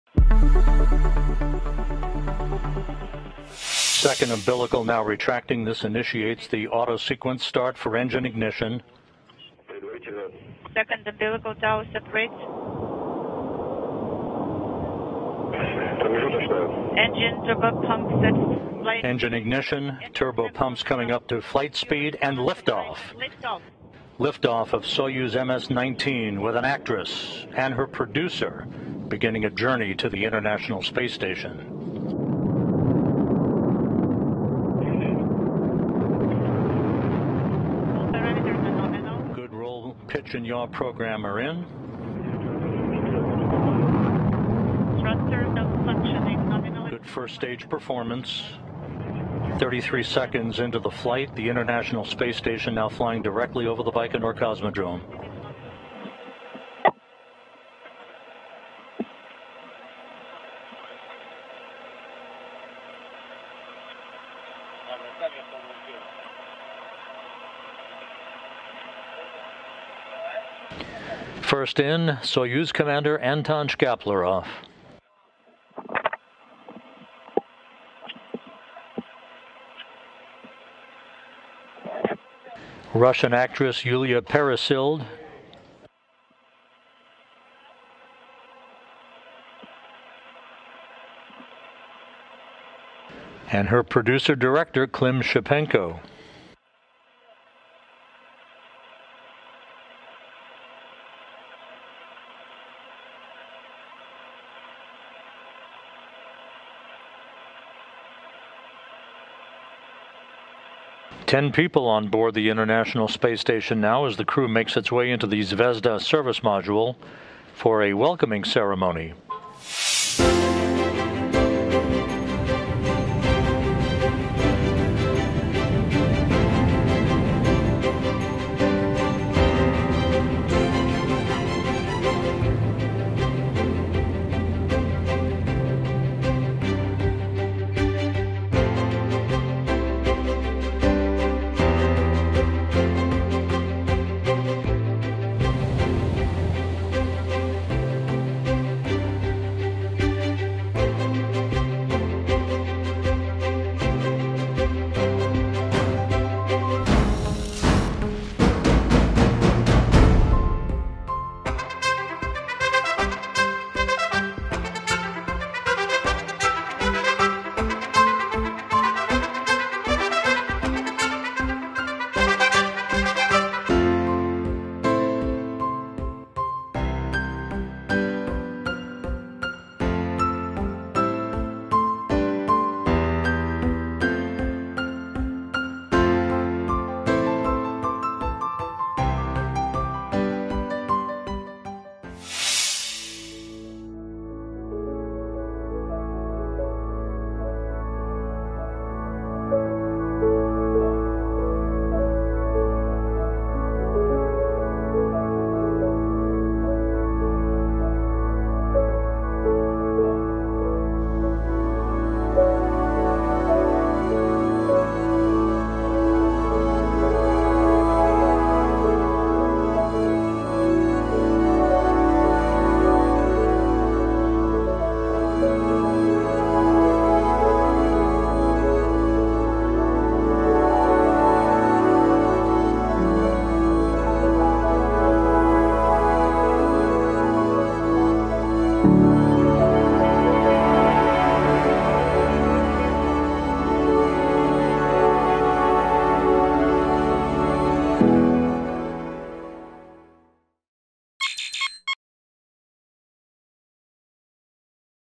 Film crew launches, 'Captain Kirk' going to space & a fireball in VFS Weekly
A Soyuz rocket launches a Russian actress and her producer to the International Space Station to film a movie. William Shatner will be on the 2nd crewed flight of Blue Origin's New Shepard rocket, and a fireball lit up the skies over Colorado.

3, 2, 1 ... action! Russia launches film crew to shoot movie on space station

William Shatner's space launch on Blue Origin's New Shepard: When to watch and what to know